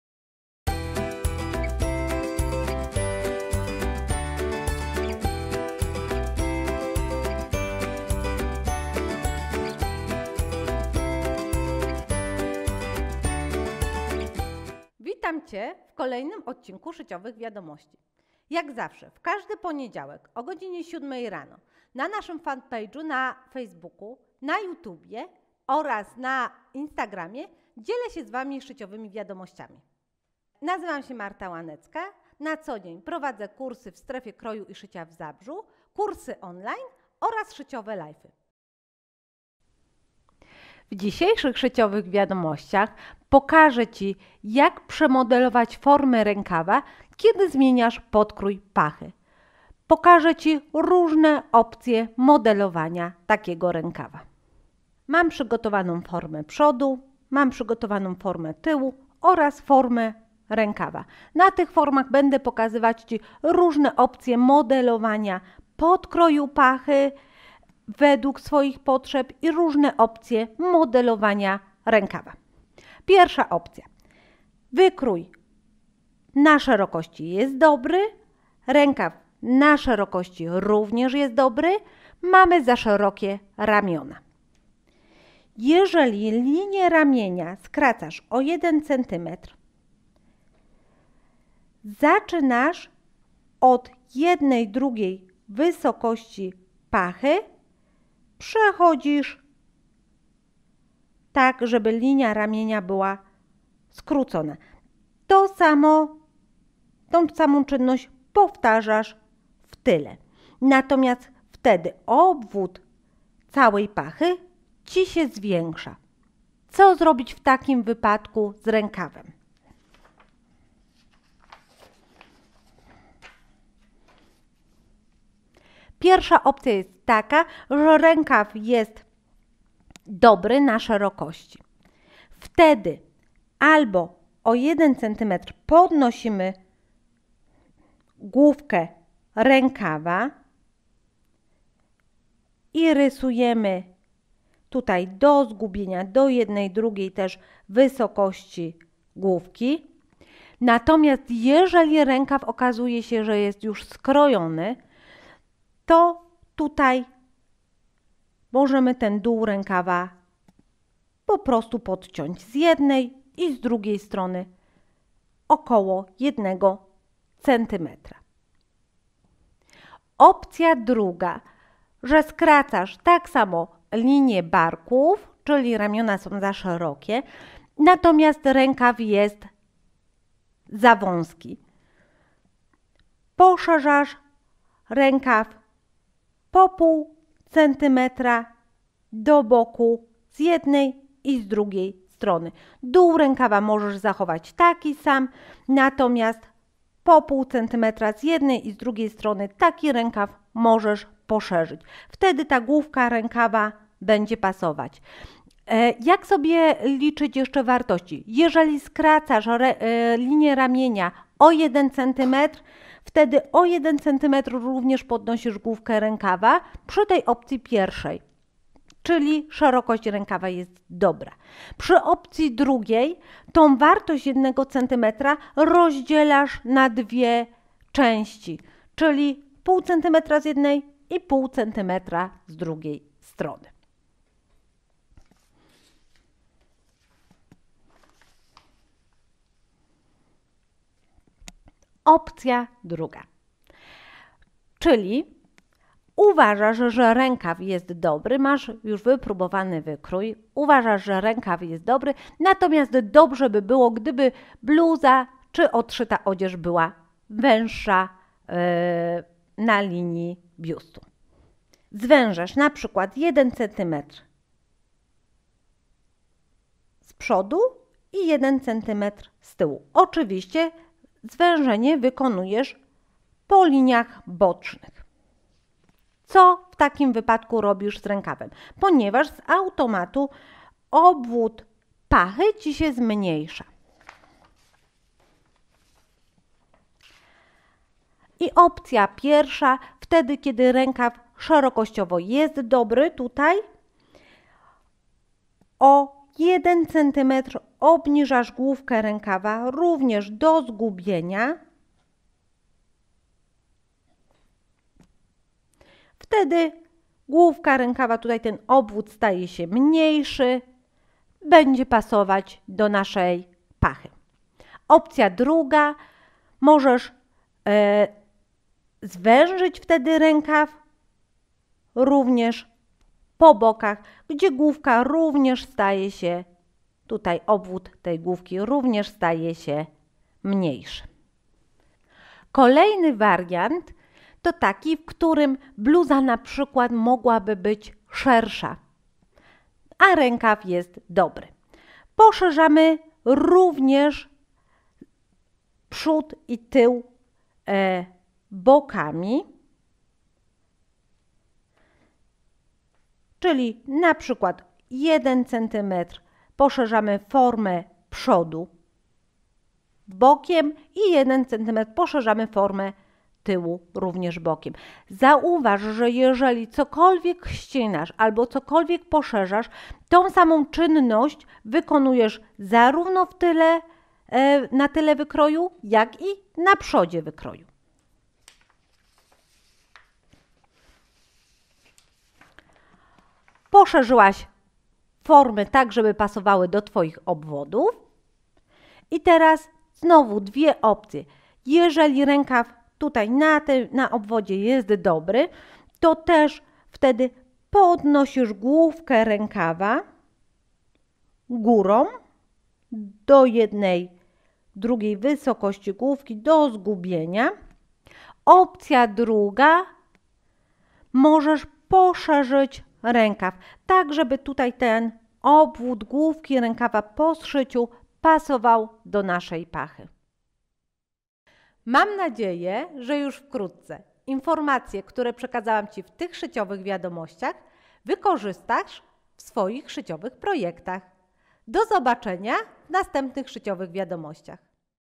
RÓŻNE OPCJE MODELOWANIA RĘKAWA
Zobacz video w którym pokazujemy jak przemodelować rękaw kiedy zmieniasz rozmiar, pokazujemy również różne opcje modelowania rękawów.
Solidna dawka wiedzy krawieckiej, triki ułatwiające szycie.

SZYCIOWE WIADOMOŚCI w każdy poniedziałek